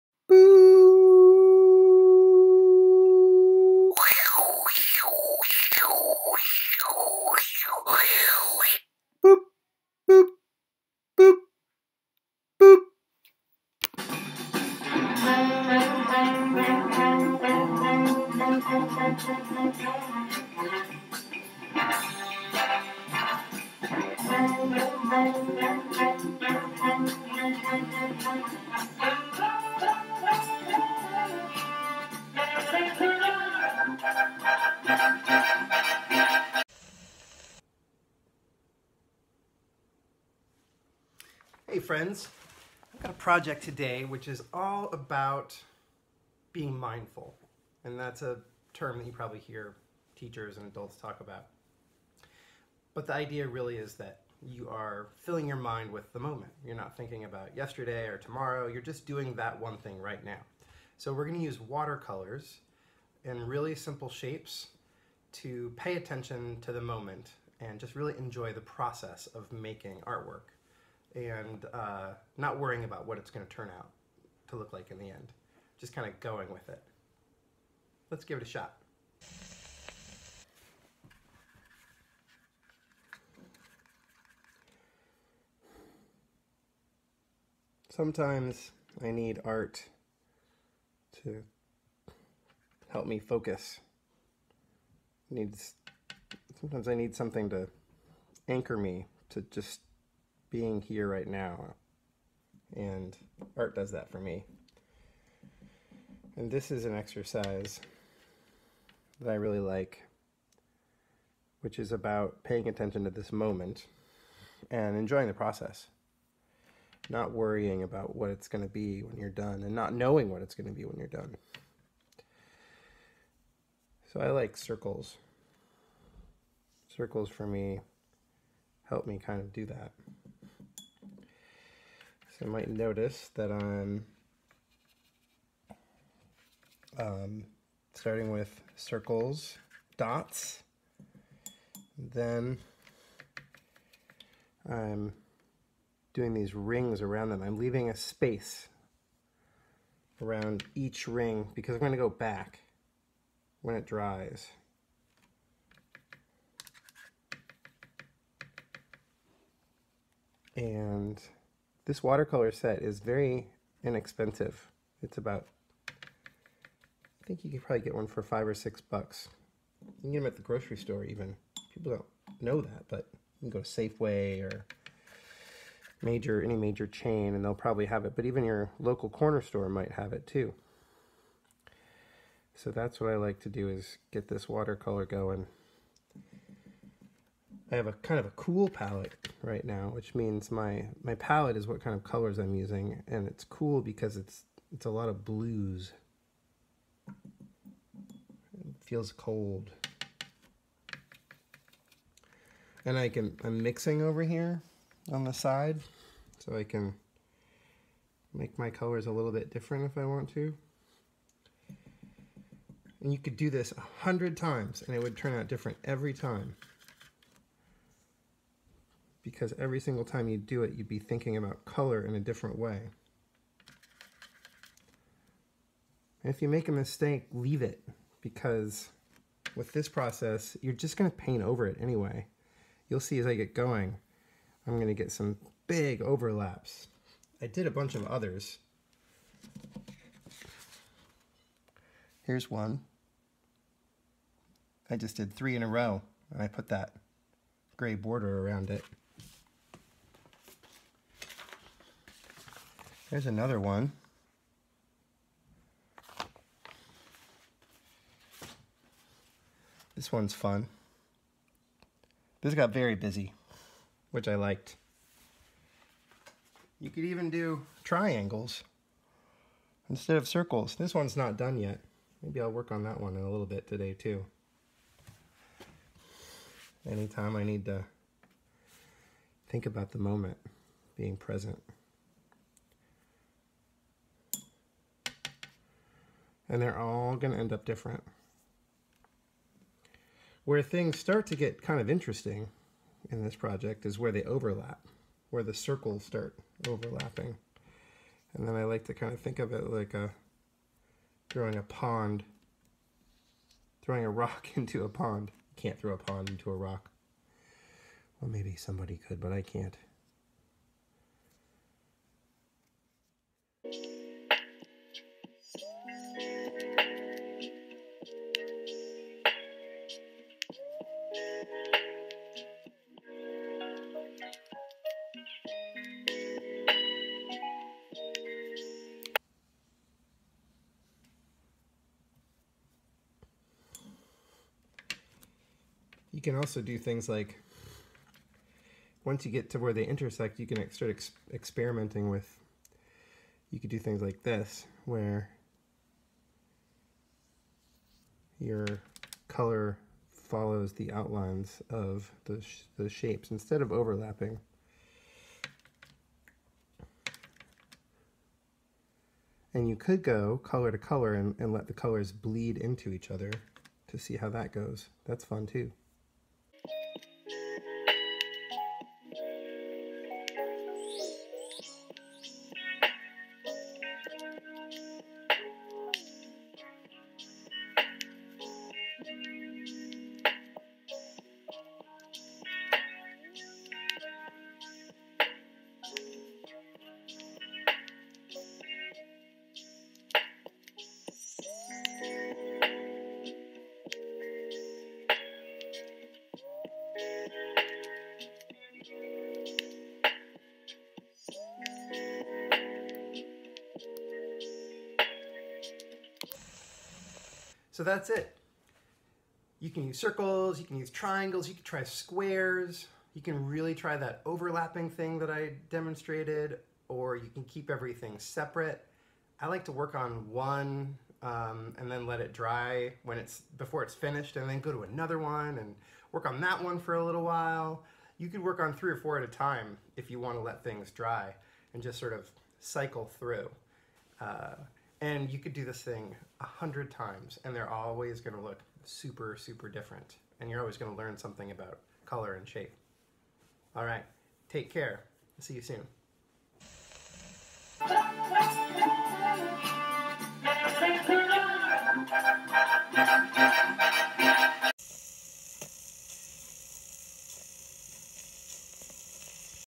Circle Mindfulness
This is a process based painting exercise to practice, color, brushwork, and staying in the moment. Give it a try!